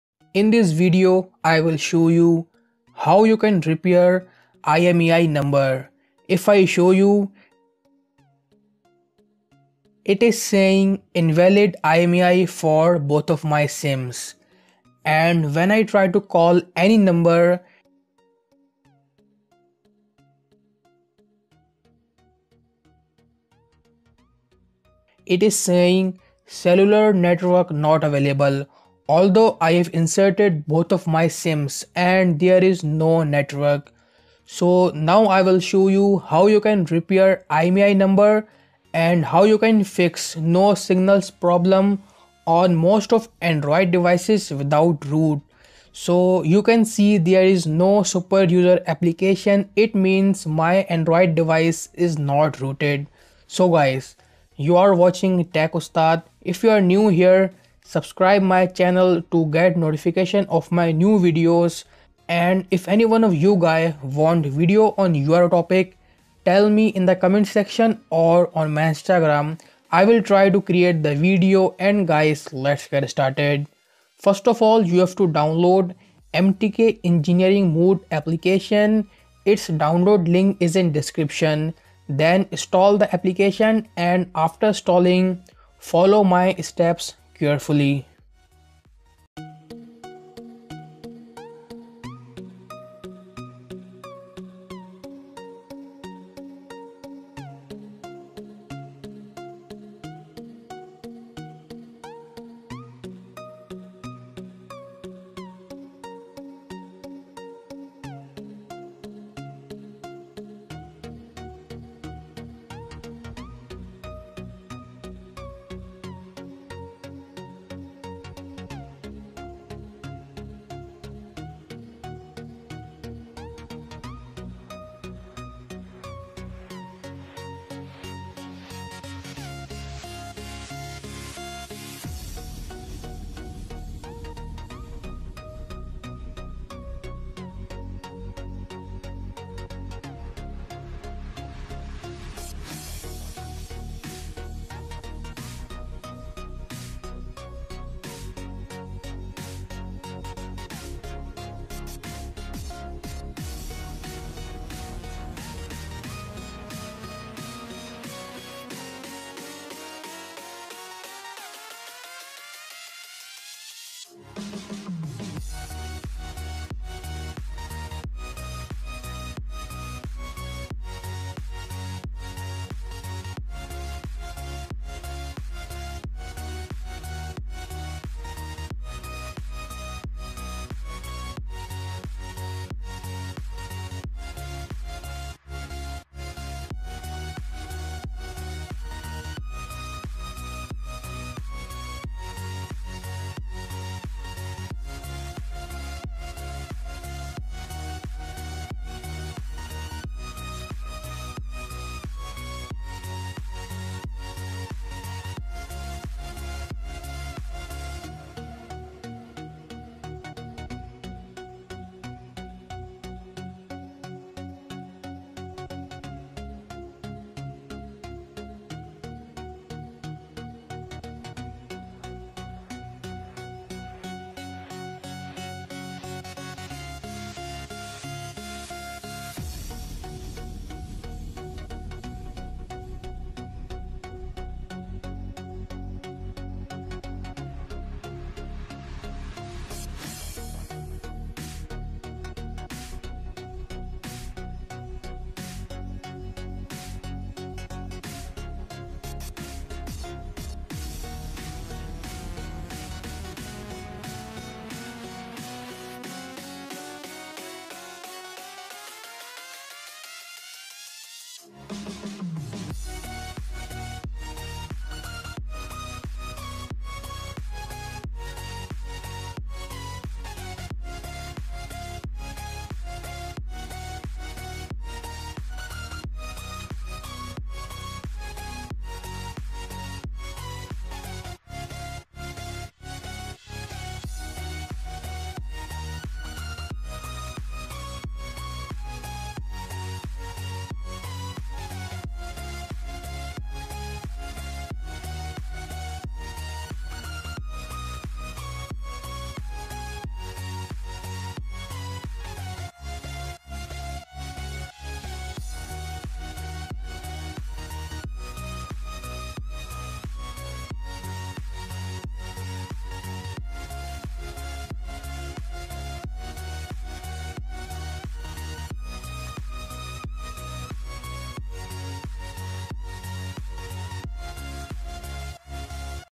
Repair IMEI Number without Root | Fix No Network | Tech Ustad
Hey guys, this video is about repairing invalid IMEI number on any Android device without root. This method will also fix your no signals/no network or not signal coming problem.

Queries solved:
1: fix invalid imei
2: fix no network issue
3: no cellular network available
4: repair imei without root
5: repair imei no pc/without pc
6: how to use mtk engineering mode.